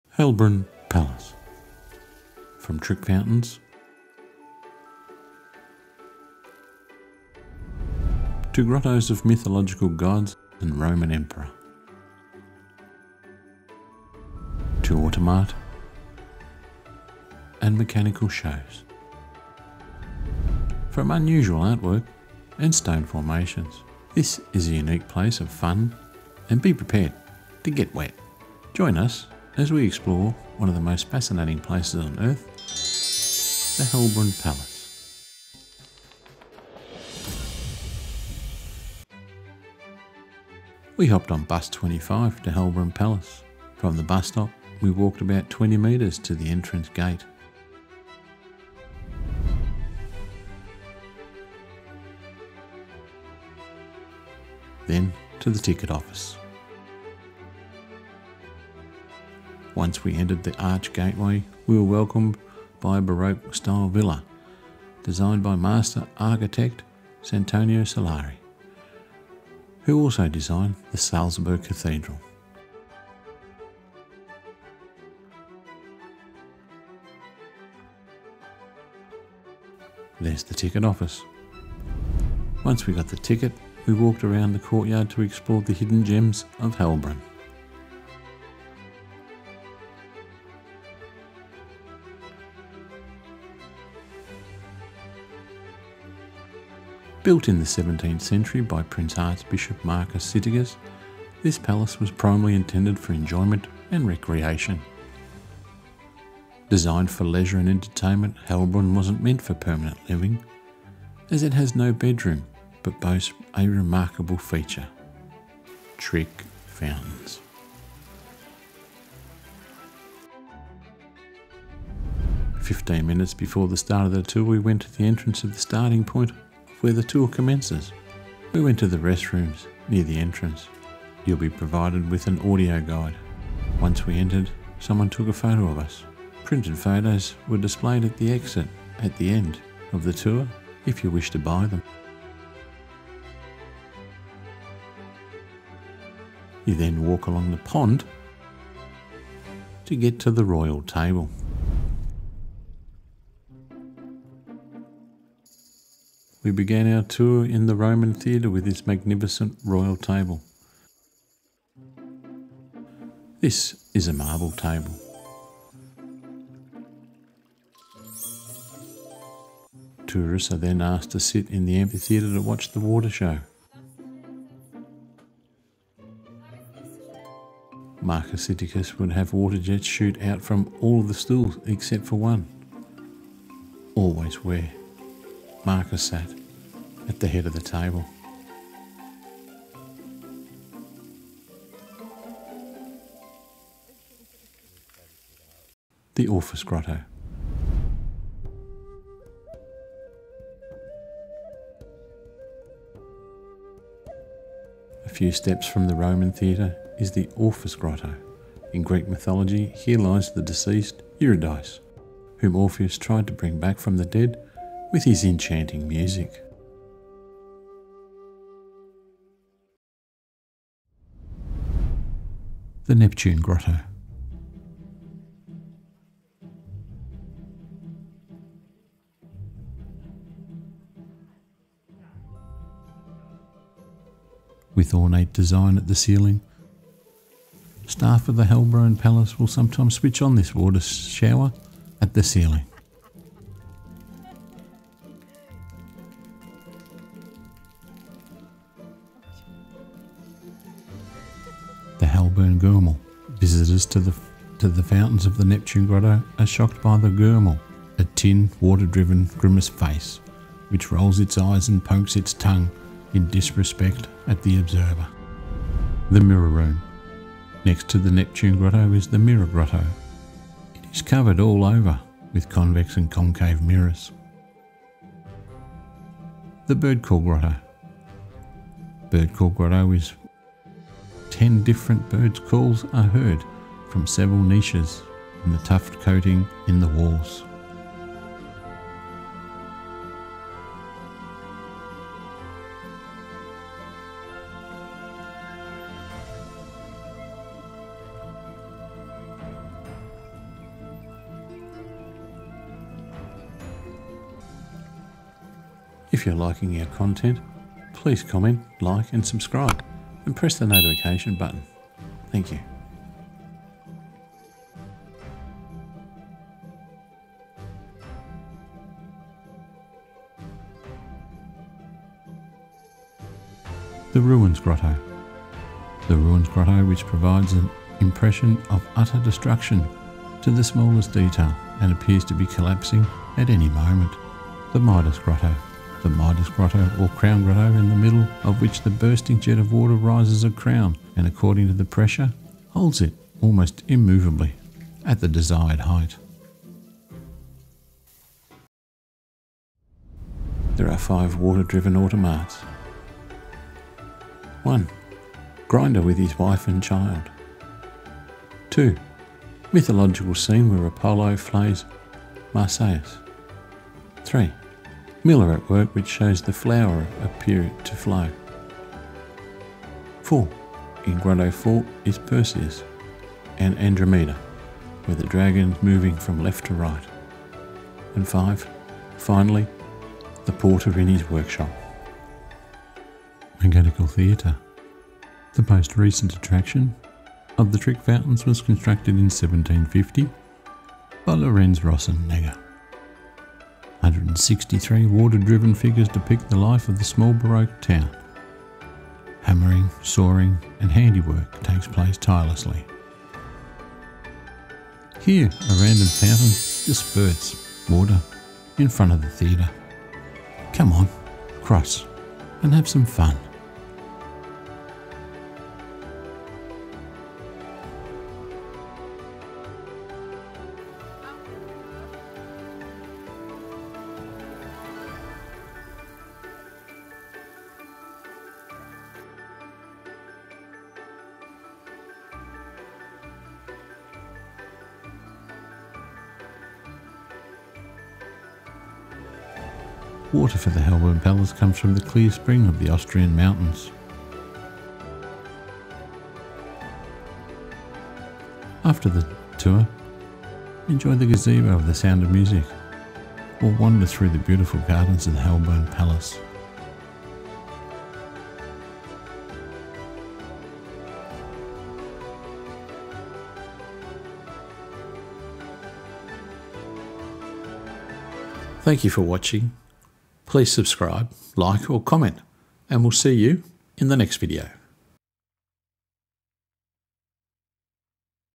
AUSTRIA | HELLBRUNN PALACE - Exploring 400-Year-Old Trick Fountains That Still Work and Hidden Gems!
Do not miss this place if you are in Salzburg! If you have a couple of hours to spare in Salzburg, Hellbrunn Palace is one of the most entertaining and fun places to visit. We took advantage of the Salzburg Card, which allowed us to enjoy the tour for free.

Hellbrunn Palace  or Schloss Hellbrunn or Hellbrunn Castle is located near Salzburg, Austria, is a 17th-century early Baroque villa that served as a summer residence for Prince-Archbishop Markus Sittikus. The mechanism behind the trick fountains which is powered by water is centuries old and still works. Be prepared to get wet and have fun!

While the castle itself is not the largest in Austria, the real highlights are the enchanting gardens, grottos, and whimsical trick fountains. It Is a delightful experience for visitors of all ages!

It is easily accessible using the #25 bus.

Hellbrunn Palace website

VIDEO TIMELINE

0:00 A Peek of What to See in Hellbrunn Palace
0:45 Walking to the Entrance and Ticket Office
2:05 Start of the Tour to the Trick Fountains
2:35 The Royal Table and Fun Tick Fountains
3:20 Orpheus Grotto
3:44 The Nepture Grotto and Water Showers!
4:17 The Hellbrunn Germaul at the foot of the Nepture Grotto
4:28 The Mirror Grotto
4:40 The Bird Call Grotto with 10 bird call sounds around the room
5:19 The Shell Grotto
5:26 The Ruins Grotto
5:40 The Crown Grotto
6:00 5 Water Automat
6:25 Mechanical Theatre in Hellbrunn Palace constructed in 1750
7:08 More trick fountains and grottos in Hellbrunn Palace

HERE’S OUR OTHER AUSTRIA VIDEOS YOU CAN BINGE WATCH AND ENJOY!